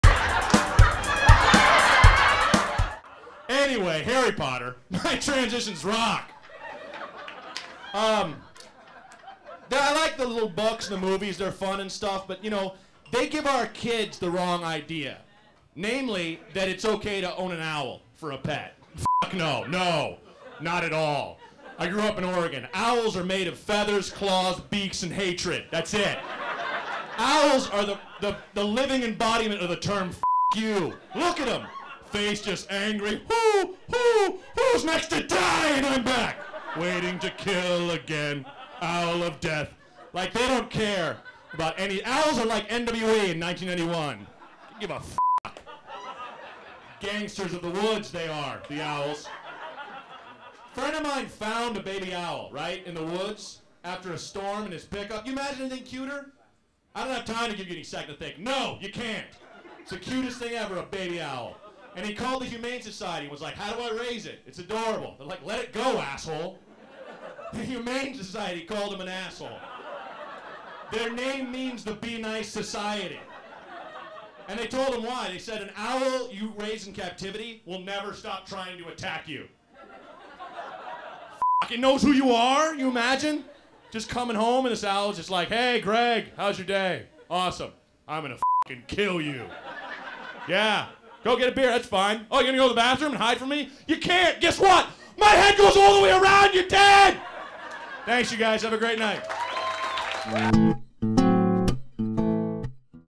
Matt Braunger - Owls With Attitude - Comedy.com
Matt warns against having owls as pets because they're the gangstas of the woods and the pure embodiment of evil!